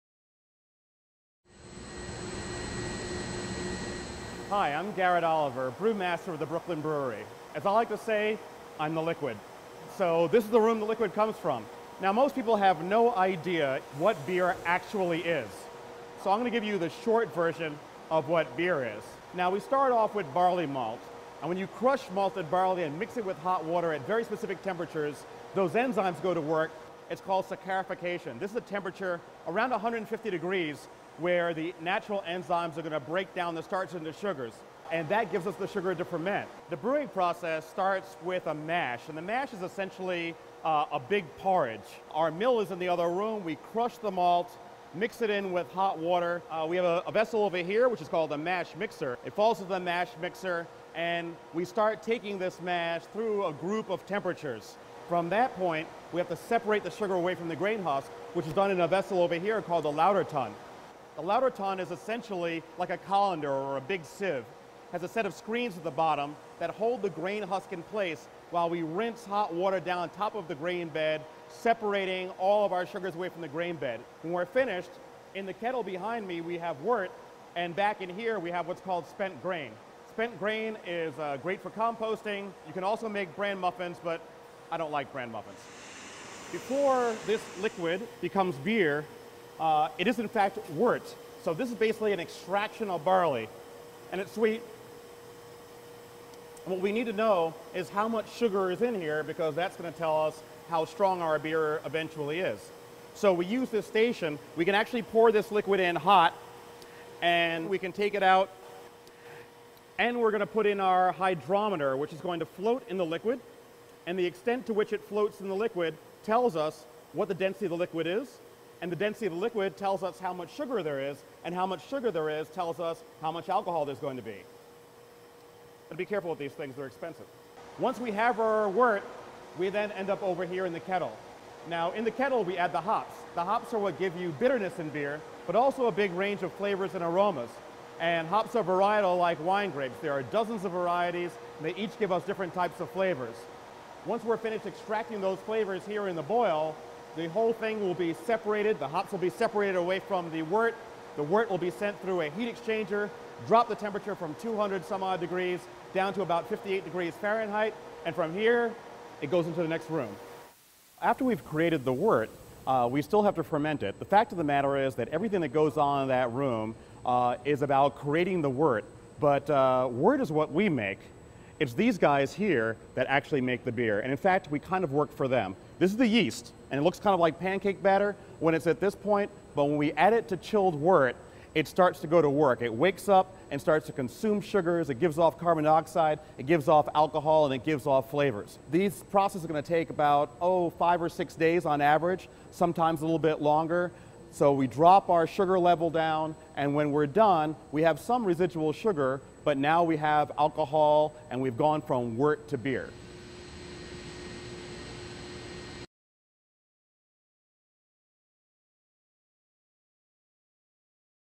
How to Make Beer - Brooklyn Brewing
Brewmaster Garrett Oliver at Brooklyn Brewing Company explains what beer is made of and how it is made!